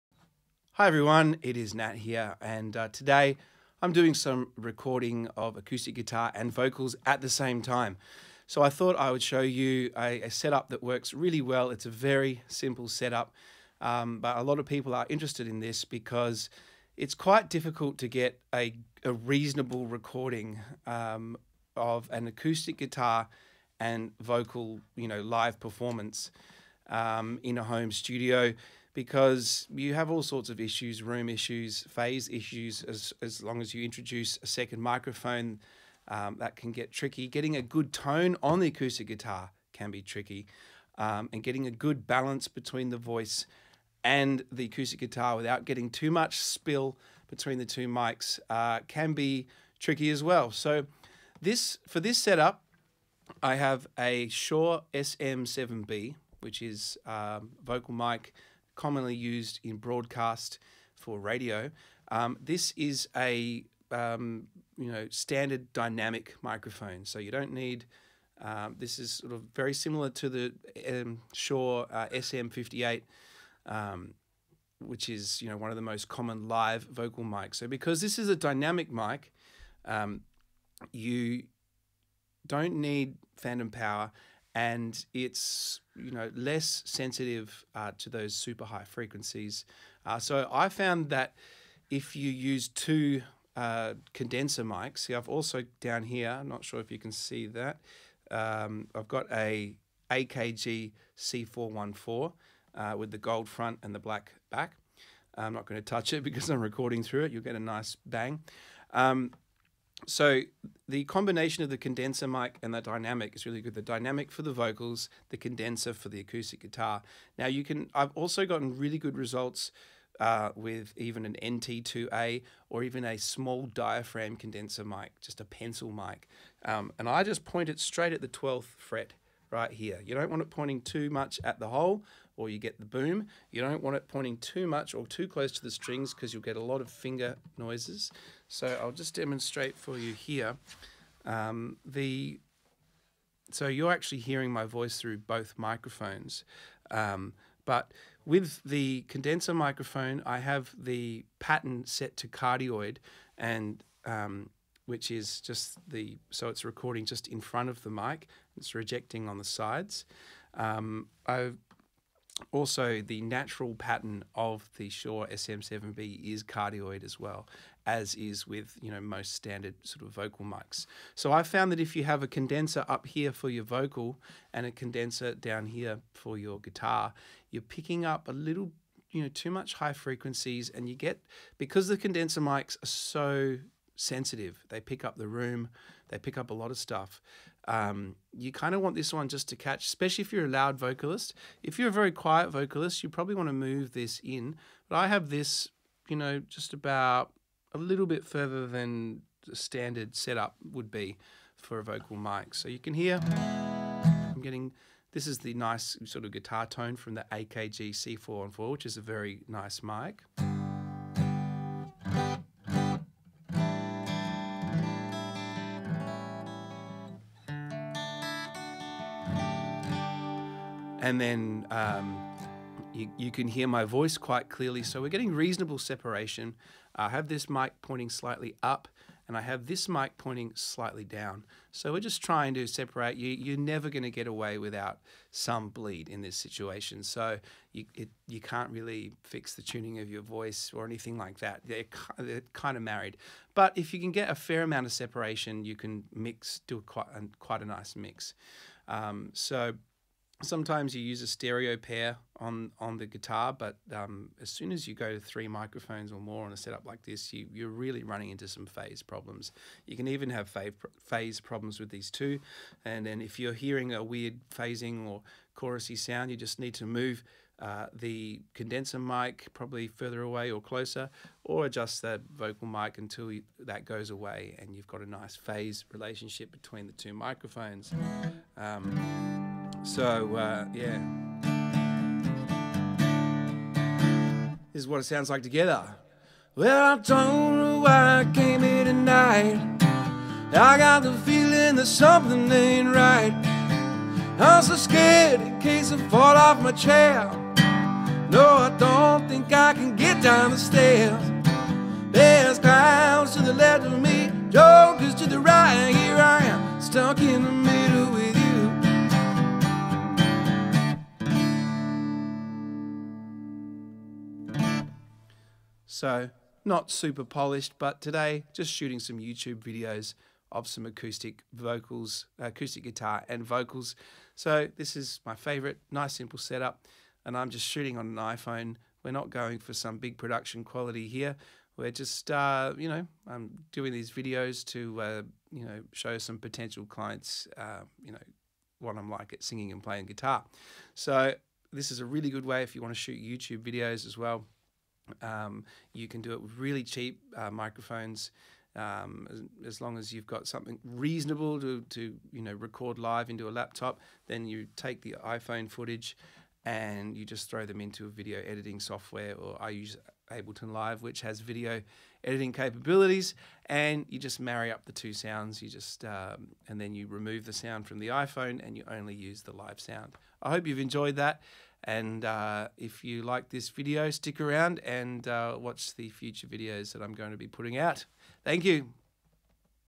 How To Record Acoustic Guitar and Vocals At The Same Time
In this video I show you a simple way to get a good sound with a two mic setup. I was recording some youtube videos and wanted a simple way to record my live performance.

Let me know if you have any other requests and I will answer them in the comments or make some new videos. - Nat